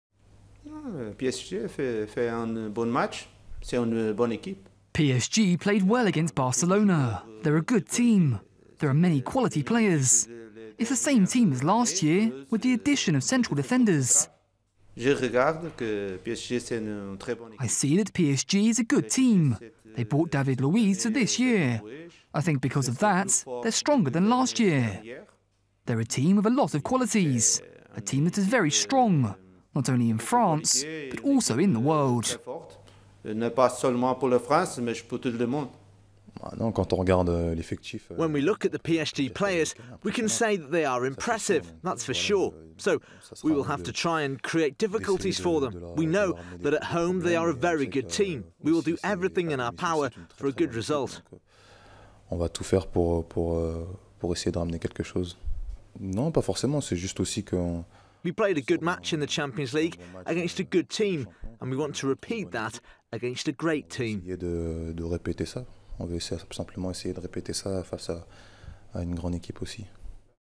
Luiz improves PSG - Jardim
Monaco coach Leonardo Jardim and defender Geoffrey Kondogbia look ahead to their Ligue 1 trip to Paris Saint-Germain.